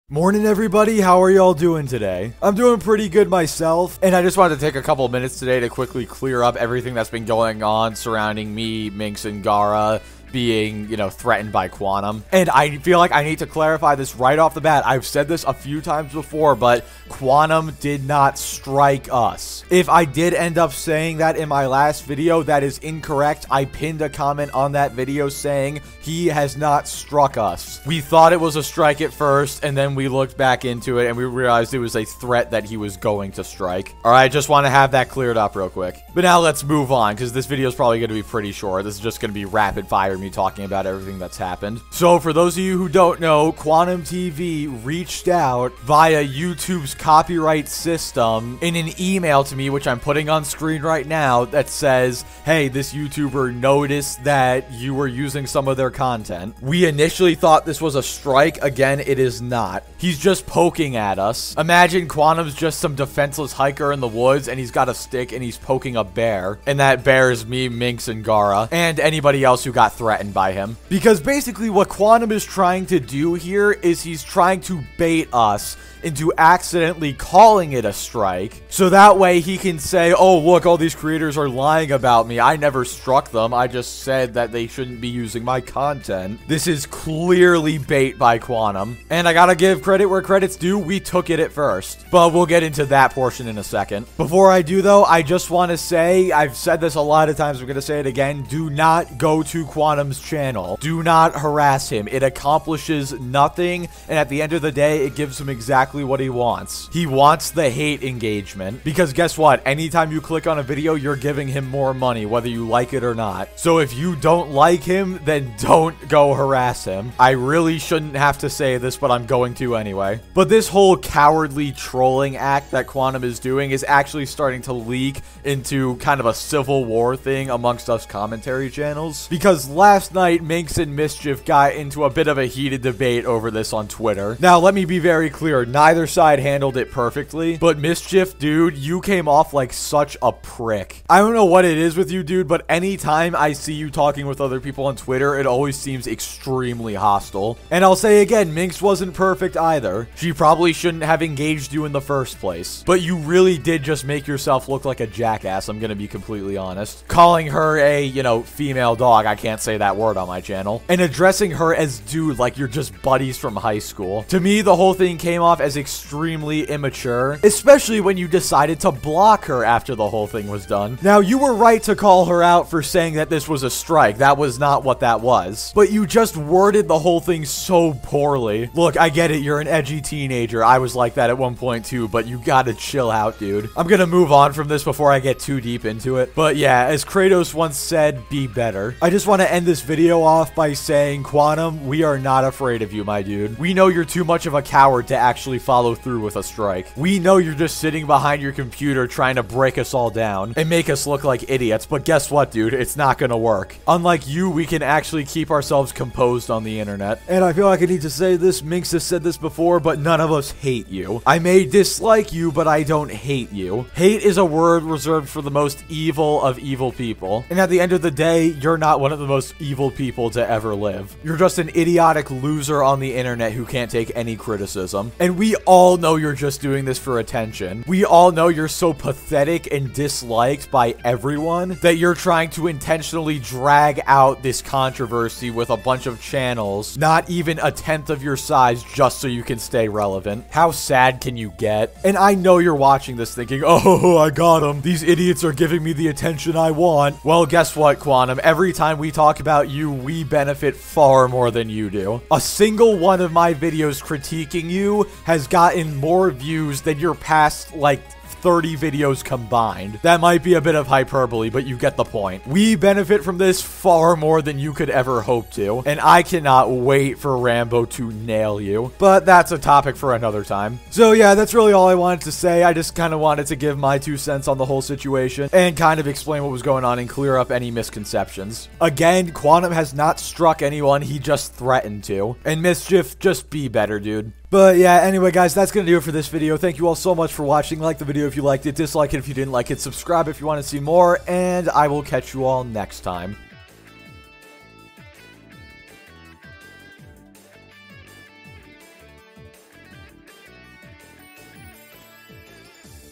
Quantum TV Threatened To Strike Me And My Friends
Just stop please...